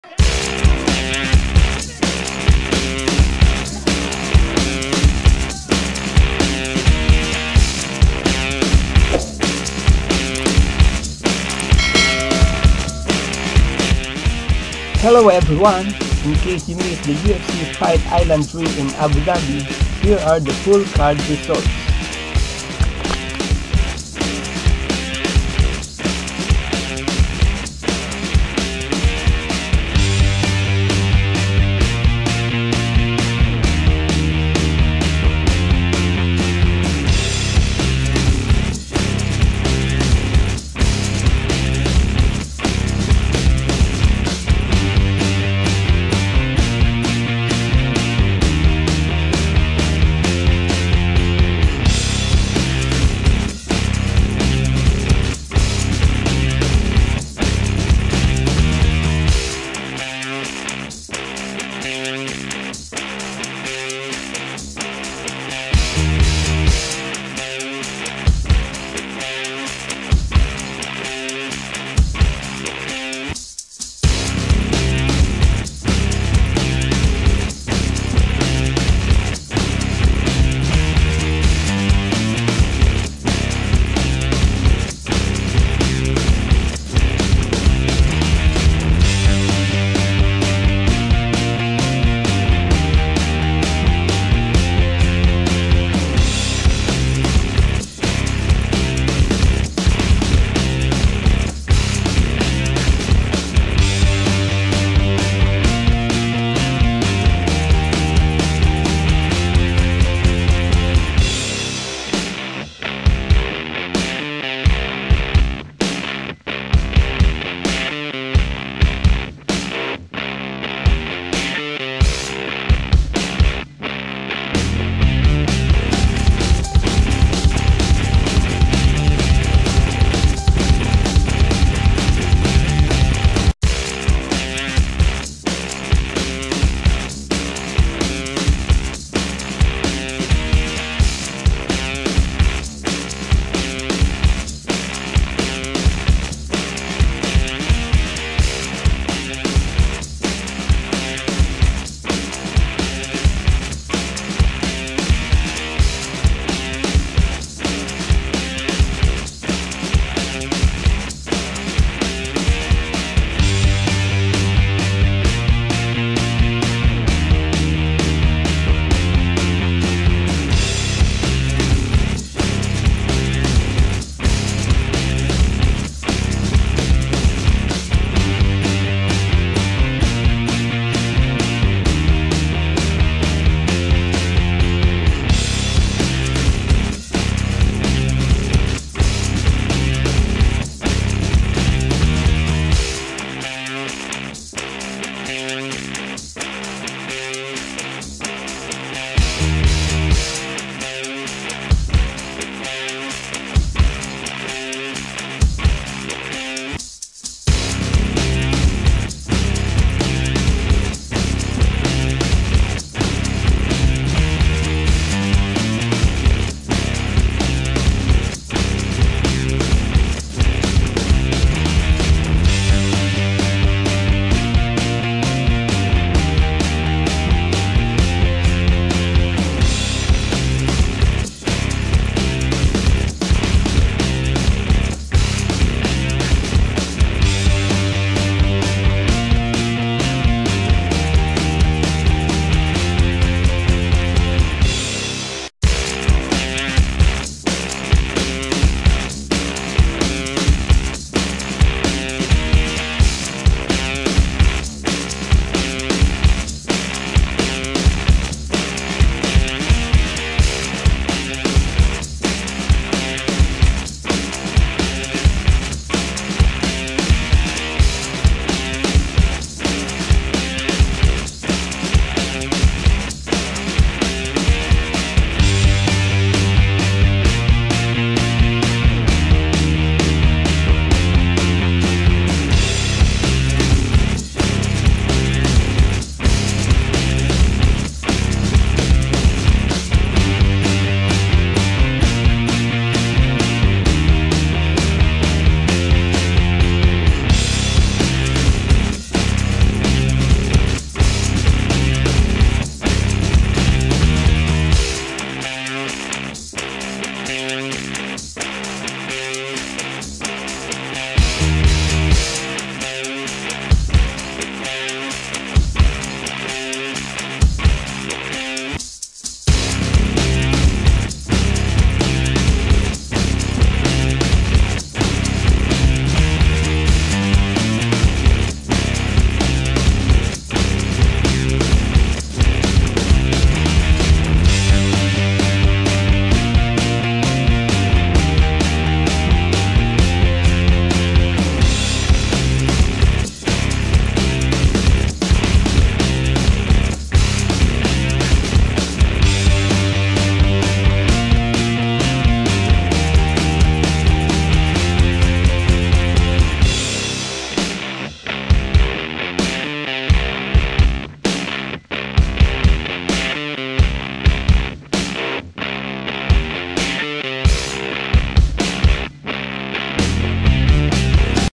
UFC FIGHT ISLAND 3 | FULL FIGHT RESULTS | JULY 25, 2020
WATCH THE COMPLETE FIGHT RESULTS HERE.

NATHANIEL WOOD DEFEATS JOHN CASTANEDA (Unanimous Decision)
RAMAZAN EMEEV DEFEATS NIKLAS STOLZE (Unanimous Decision)
PANNIE KIANZAD DEFEATS BETHE CORREIA (Unanimous Decision)
TANNER BOSER DEFEATS RAPHAEL PESSOA BY TKO AT 2:36 IN ROUND 2
MOVSAR EVLOEV MOVSAR EVLOEV DEFEATS MIKE GRUNDY BY UNANIMOUS DECISION
TOM ASPINALL DEFEATS JAKE COLLIER BY TKO AT :45 IN ROUND 1
JESSE RONSON DEFEATS NICOLAS DALBY BY SUBMISSION VIA REAR NAKED CHOKE AT 2:48 IN ROUND 1
KHAMZAT CHIMAEV DEFEATS RHYS MCKEE BY TKO AT 3:09 IN ROUND 1
ALEX OLIVEIRA (30-27 X 3) DEFEATS PETER SOBOTTA BY UNANIMOUS DECISION.
PAUL CRAIG DEFEATS PETER GADZHIMURAD ANTIGULOV BY SUBMISSION DUE TO A TRIANGLE CHOKE AT 2:06 IN ROUND
CARLA ESPARZA DEFEATS MARINA RODRIGUEZ BY SPLIT DECISION
FABRICIO WERDUM DEFEATS ALEXANDER GUSTAFSSON BY SUBMISSION DUE TO AN ARMBAR AT 2:30 IN ROUND 1
MAURICIO RUA (29-28 X 2) DEFEATS ANTONIO ROGERIO NOGUEIRA (29-28) BY SPLIT DECISION
ROBERT WHITTAKER BEAT DARREN TILL.